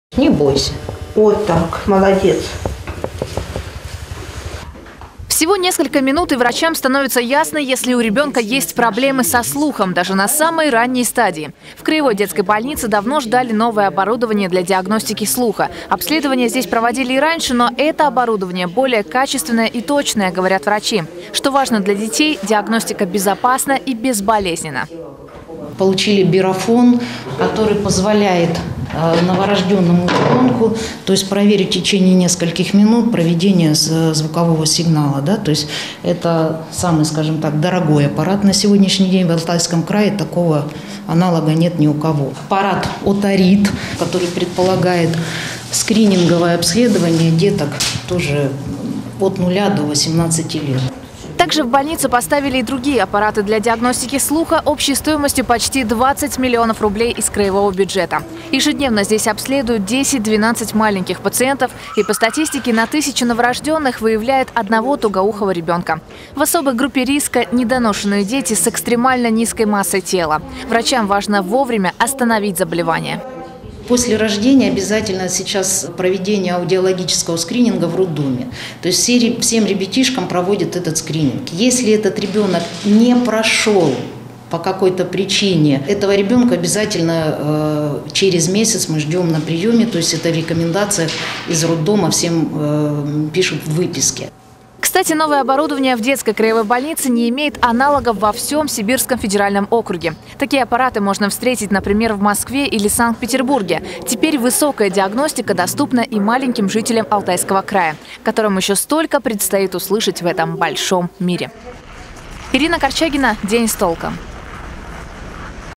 Алтайские врачи смогут на ранней стадии выявлять и лечить нарушения слуха у детей
Выявить самые серьёзные проблемы со слухом, а главное, вовремя принять меры. В центре охраны материнства и детства закупили новое оборудование стоимостью – 20 миллионов рублей. Что говорит статистика, и почему важно проводить диагностику слуха у новорождённых - смотрите в сюжете.